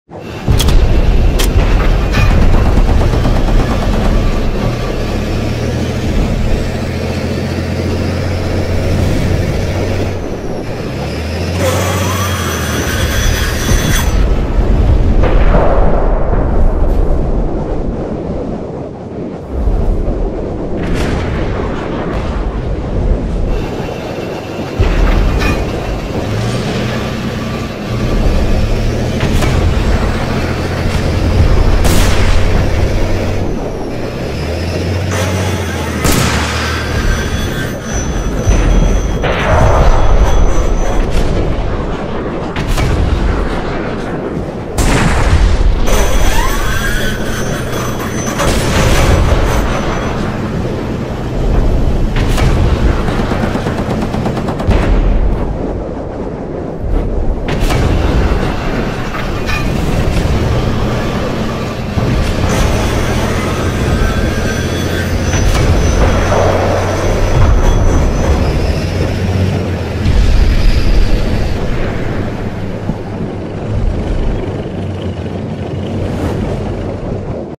Ukraynalı tankçılar Rus mevzilerine girip piyadeleri vurdu
Mariyinka, şehrinde T-64BV kullanan tankçı hızla düşman piyadelerin mevzilerine hücum etti. Evlerin yıkıntıları arasına gizlenmiş Rus piyadelerinin neredeyse tamamını etkisiz hale getirdi. Tank mürettebatının cesaretinin harika bir örneği!